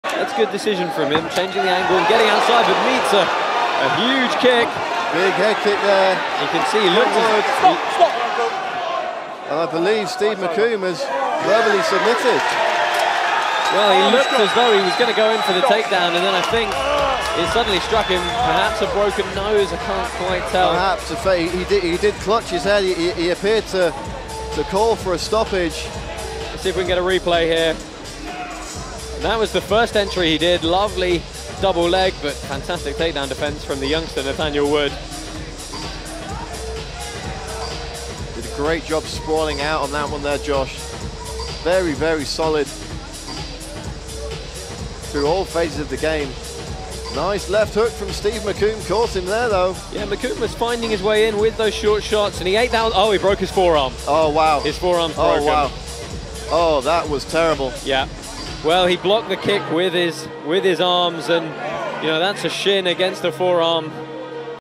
Cage Warriors 74: Nathaniel Wood defeats Steve McCombe via verbal submission
Cage Warriors 74: Nathaniel Wood defeats Steve McCombe via verbal submission - London, England (November 15, 2014).